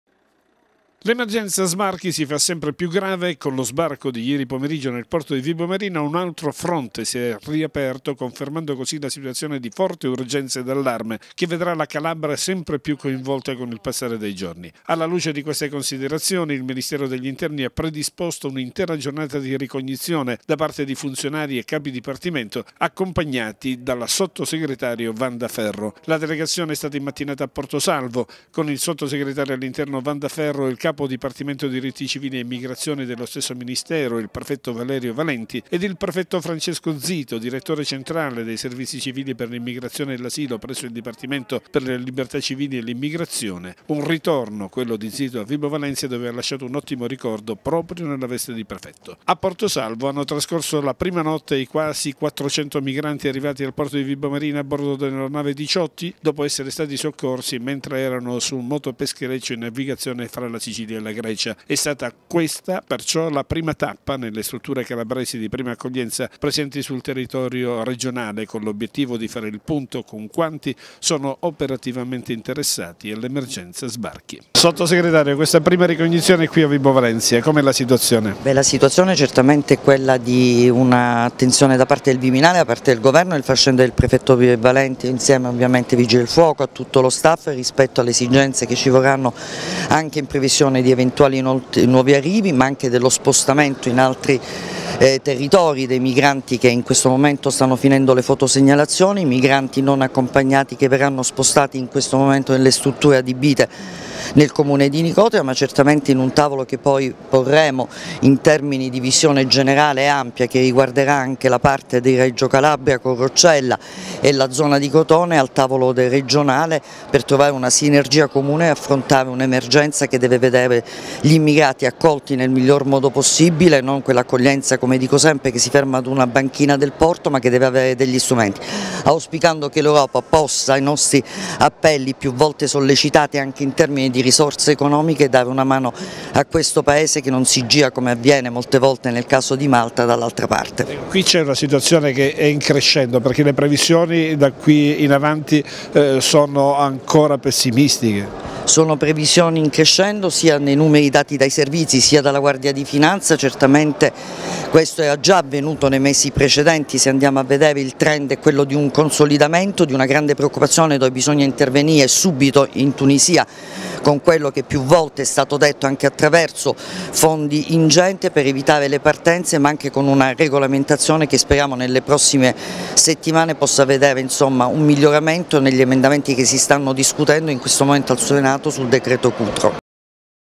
A Portosalvo Wanda Ferro e il capo dipartimento Immigrazione del Ministero Valenti
Il sottosegretario all'Interno Wanda Ferro, il Capo  dipartimento Diritti civili e Immigrazione del ministero dell'Interno, Valerio Valenti ed al prefetto Francesco Zito, Direttore Centrale dei servizi civili per l'immigrazione e l'asilo presso il Dipartimento per le libertà civili e l'immigrazione sono oggi in Calabria per fare il punto con i prefetti e i soggetti istituzionali interessati dall'emergenza sbarchi, prima tappa Portosalvo a Vibo Valentia dove sono stati sistemati i migranti sbarcati ieri sera dalla Diciotti.

Intervista Wanda Ferro, sottosegretario all'Interno